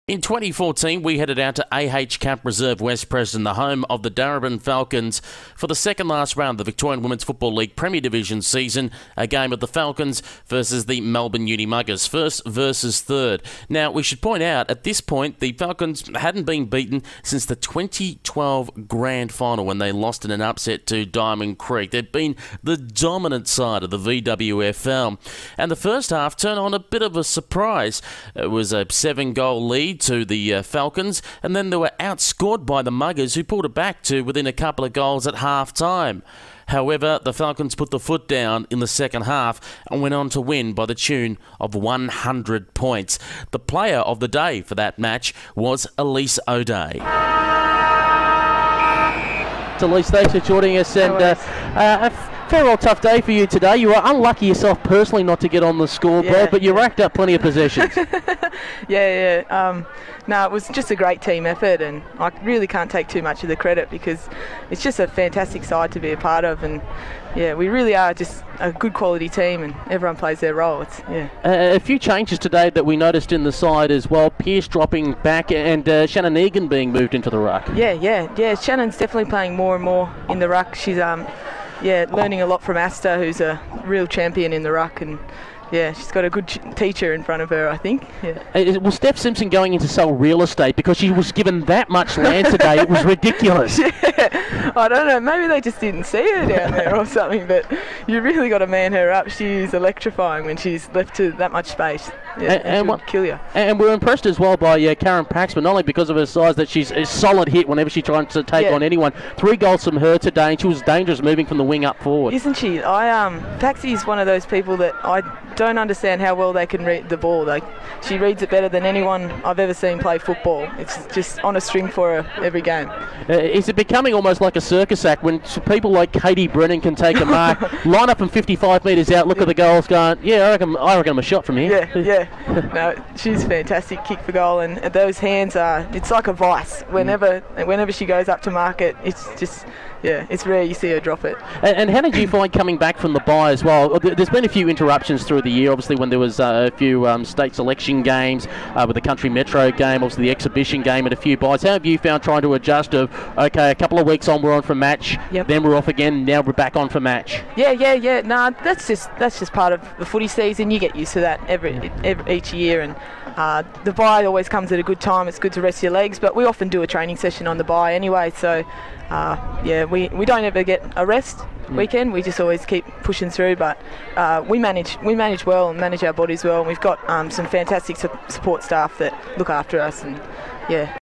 Before they were AFLW stars - Elise O'Dea
Originally recorded August 2014 with then Darebin Falcons (VWFL) midfielder, now Melbourne Demons (AFLW) midfielder, Elise O'Dea.

Aired during the Women's Australian Rules Football Radio (then VWFL Radio) post-match coverage of the Darebin Falcons v Melbourne University VWFL match at A.H. Capp Reserve.